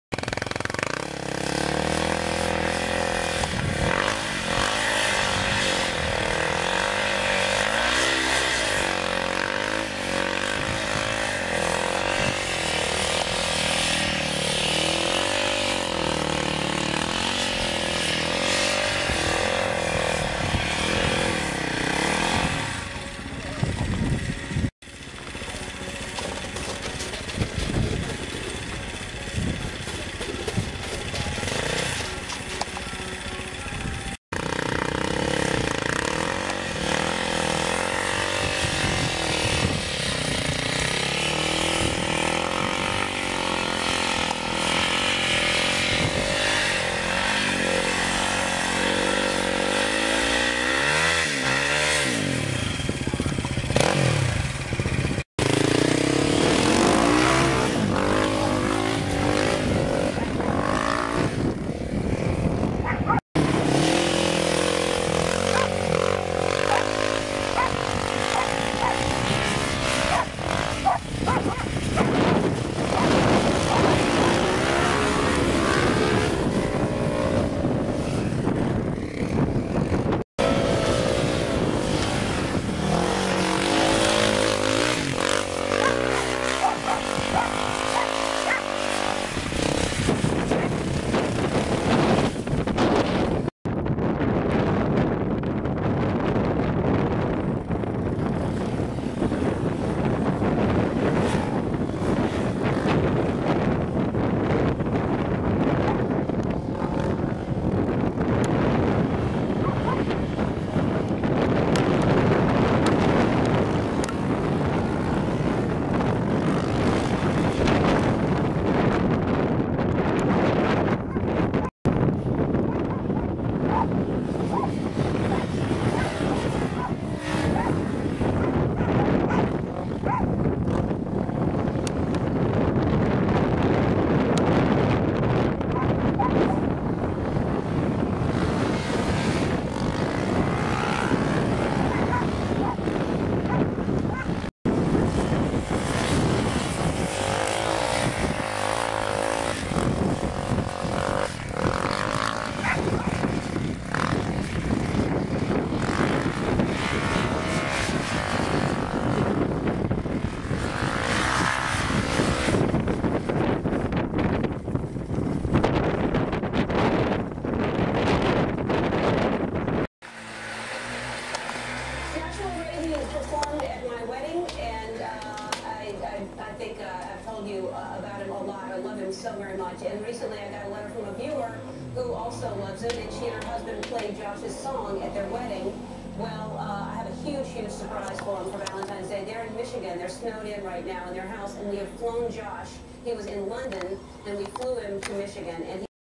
DS 450 in snow
Running my DS 450 in the Nebraska snow.